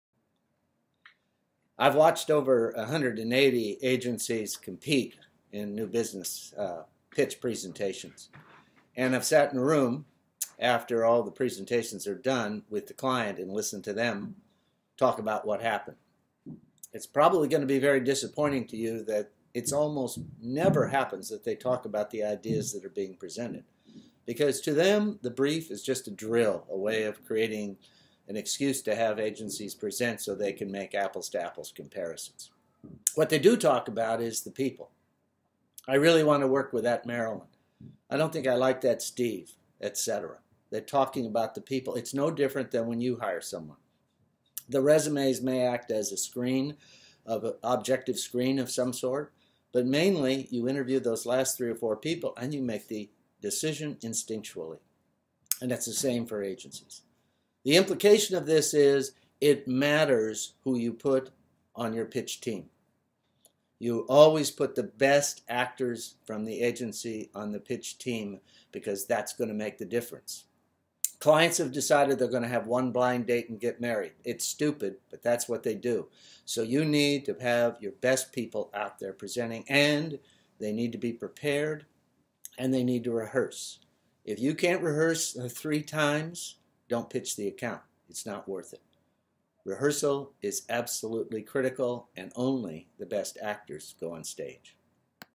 What happens at the client decision-making conference room table.
You’ll disappointed to learn that it’s unusual for the clients to ever talk about the ideas presented. Here's what they focus on.